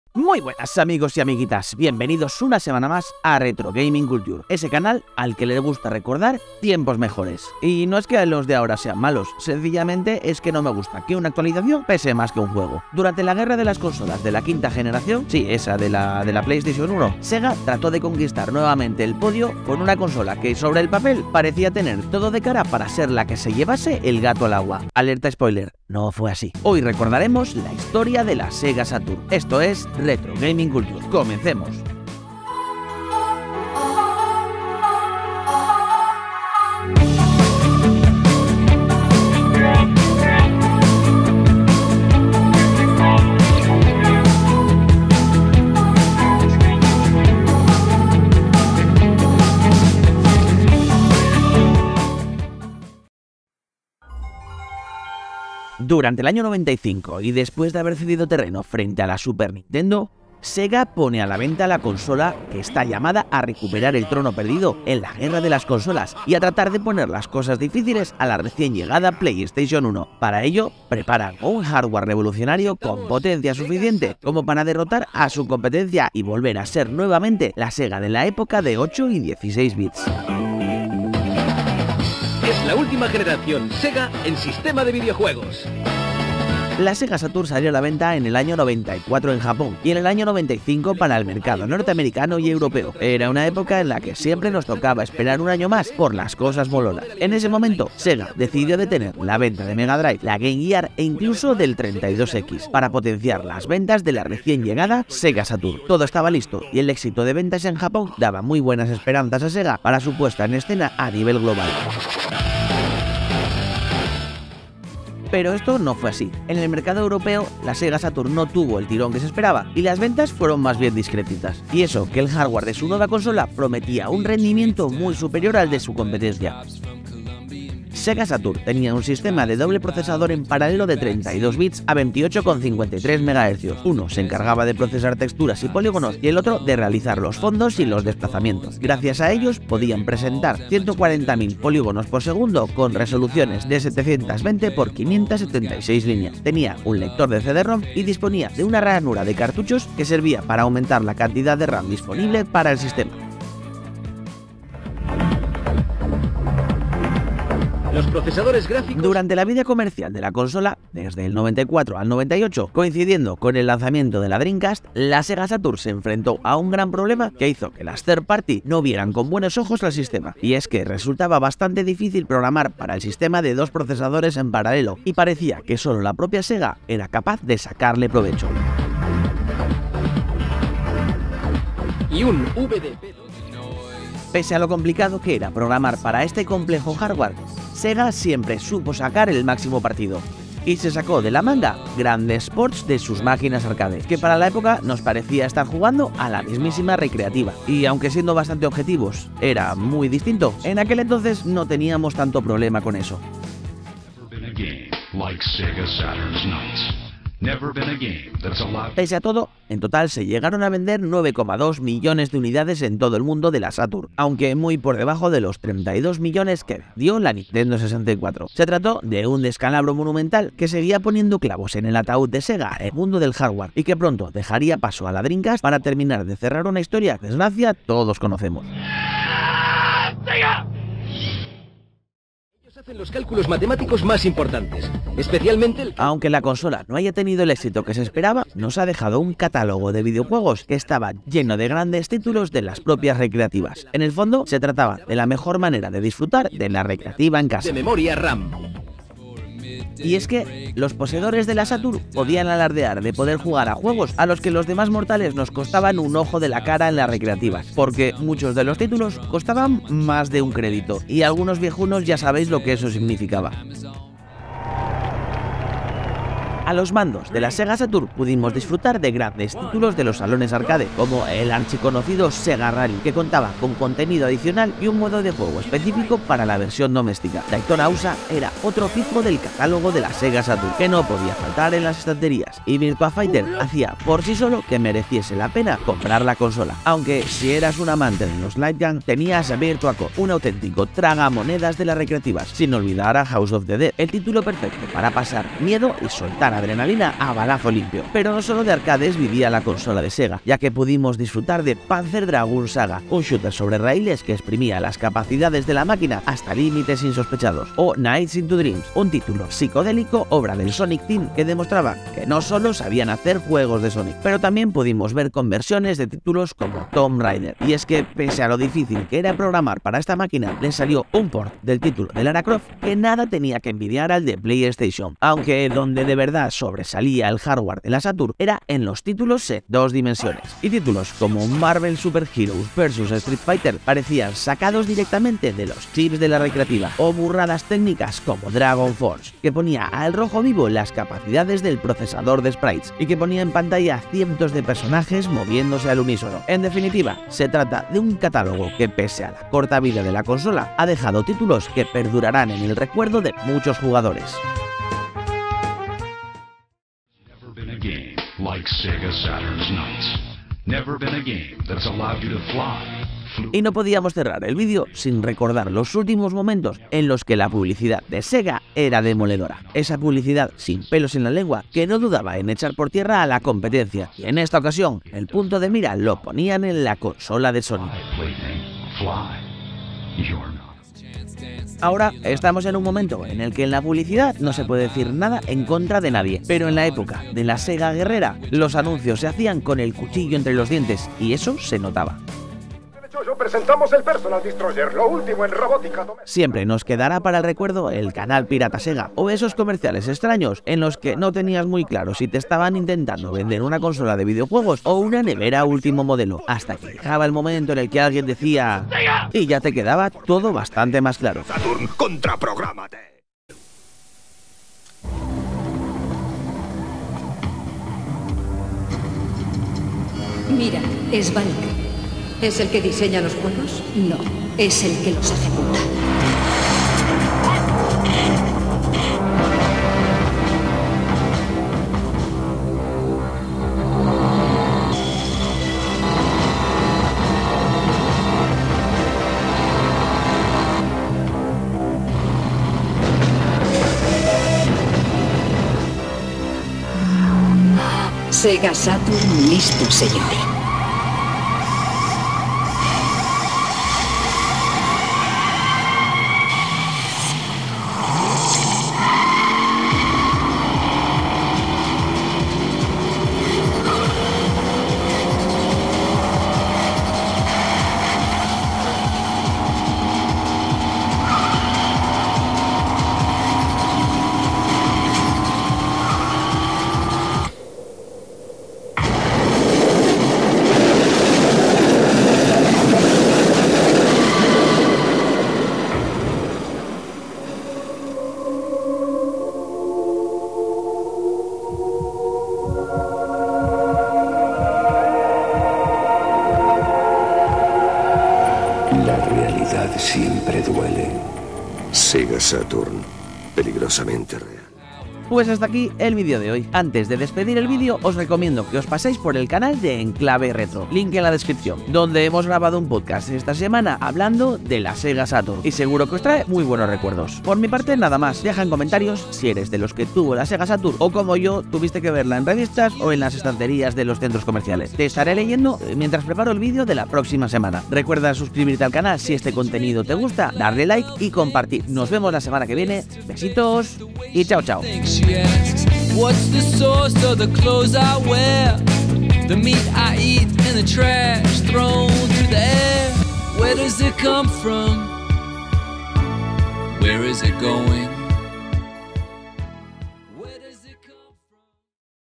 El gran DESASTRE de SEGA, Sega Saturn
La Sega Saturn fue una consola incomprendida, y que terminó en desastre, aunque merecía haber vendido más, por la cantidad de juegazos que nos dejó.